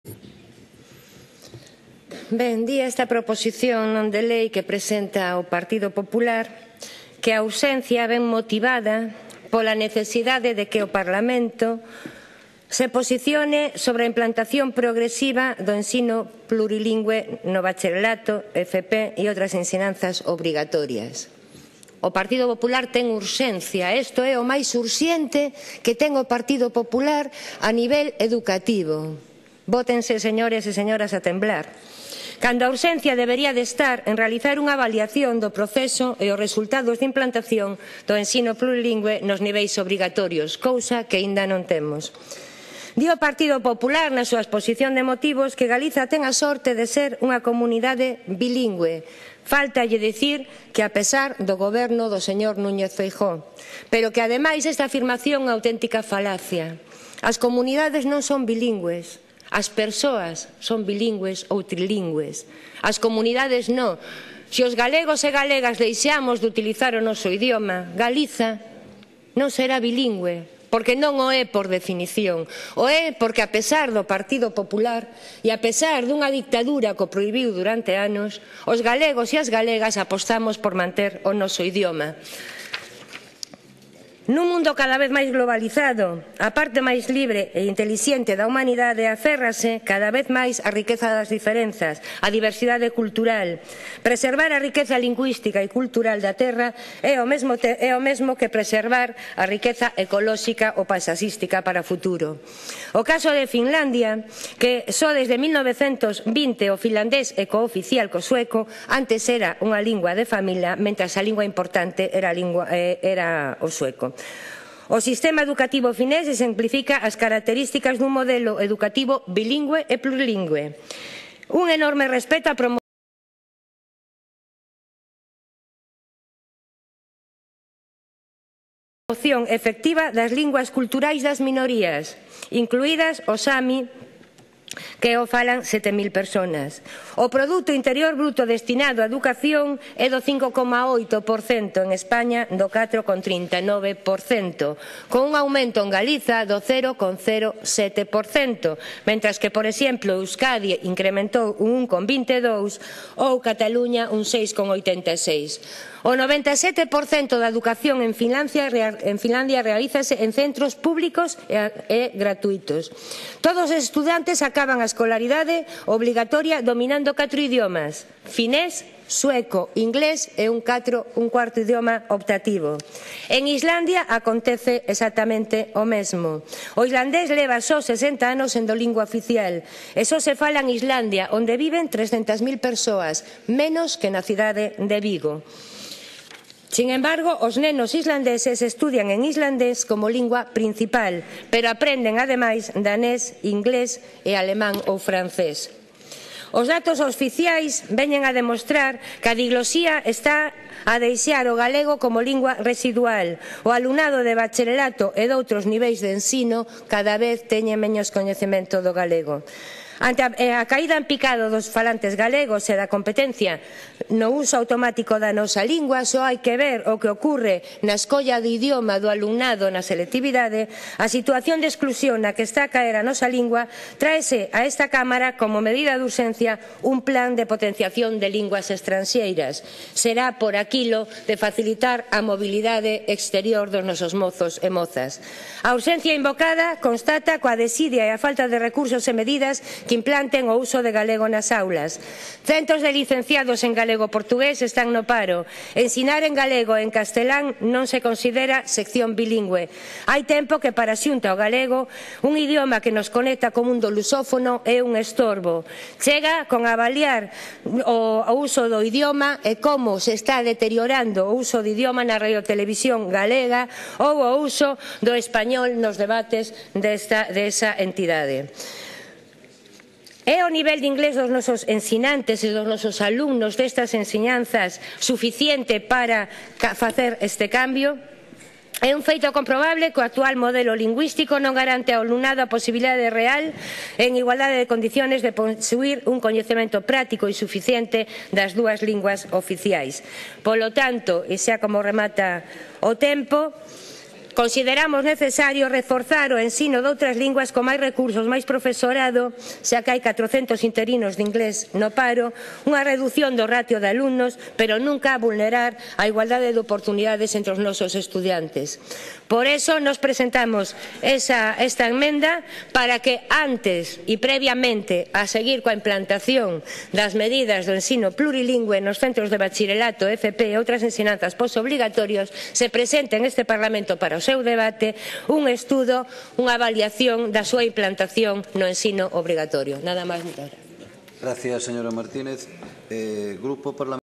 O PP apela ao plurilingüísmo para reducir horas de ensinanza en galego
Cómpre reforzar o ensino de outras linguas, pero nunca vulnerar a igualdade de oportunidades das persoas galegofalantes.
O galego perde falantes entre a mocidade, pero para o PP o prioritario é implantar un suposto modelo de plurilingüísmo nas ensinanzas medias. As comunidades non son bilingües, sono as persoas, e para que a nosa mocidade sexa plurilingüe o que cómpre é máis fondos e recursos educativos para a ensinanza de linguas, non facer demagoxia apelando ao plurilingüísmo para reducir as horas de ensinanza en galego.
Antes de seguir coas medidas do ensino plurilingüe nas ensinanzas medias, cómpre que se presente neste Parlamento un estudo sobre os resultados da implantación do suposto plurilingüísmo na ensinanza primaria.